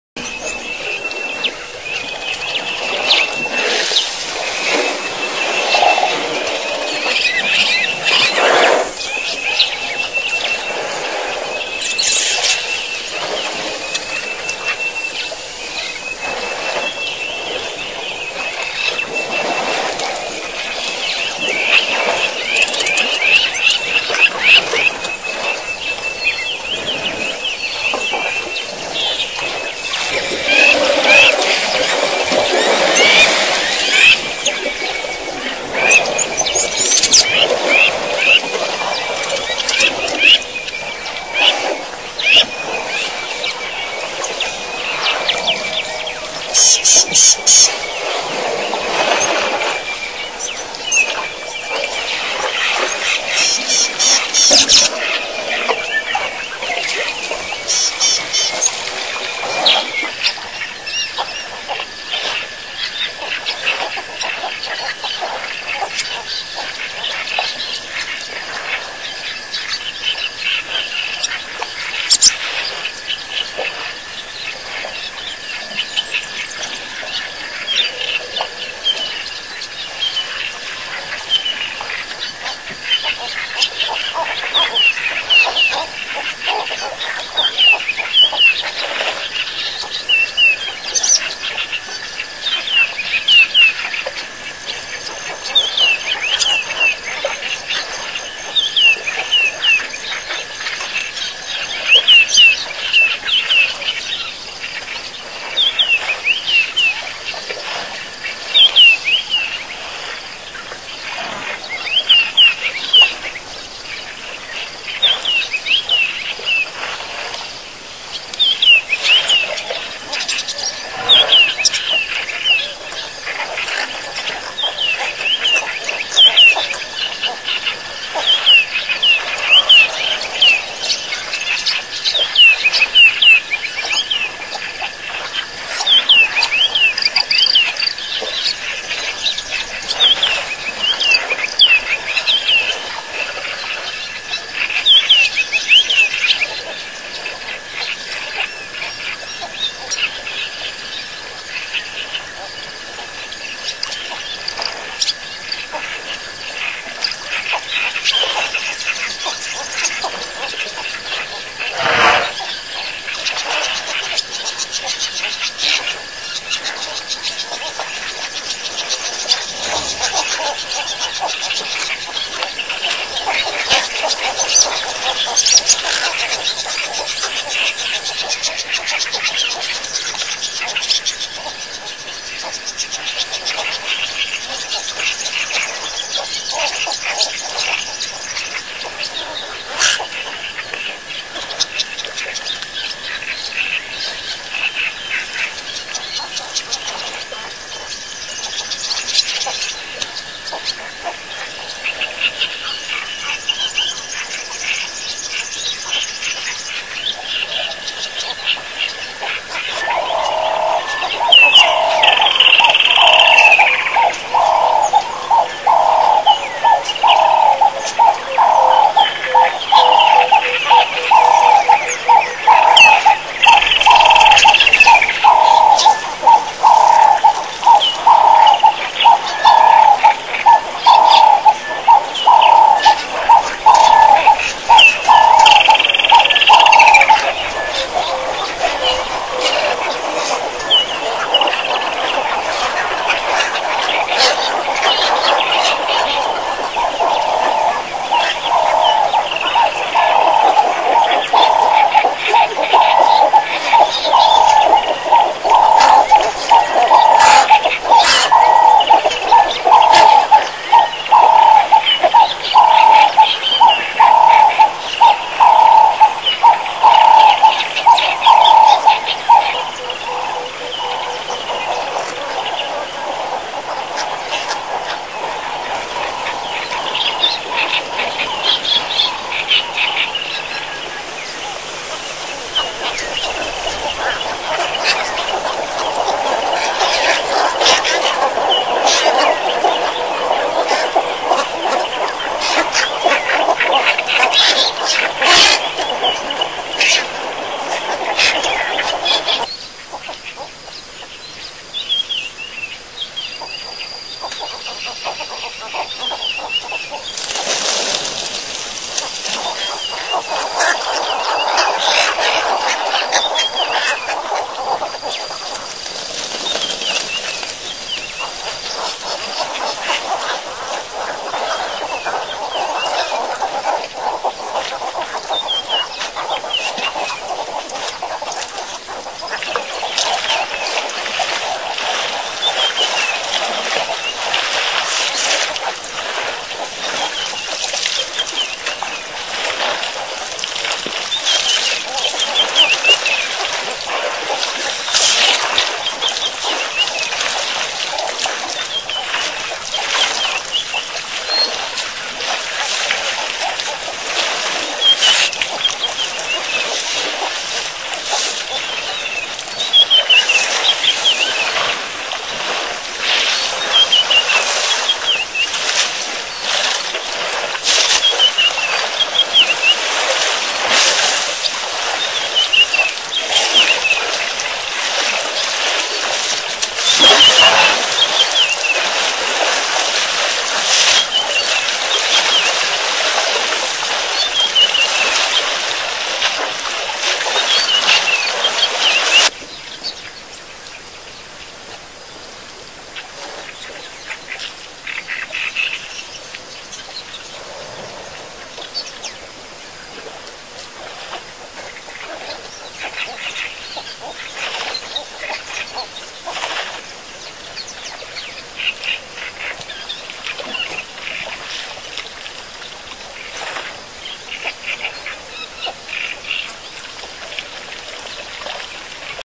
Elephants at Pete's Pond
Elephant herds at Pete's Pond - includes adult female with healed broken leg, ellies having fun chasing off egrets.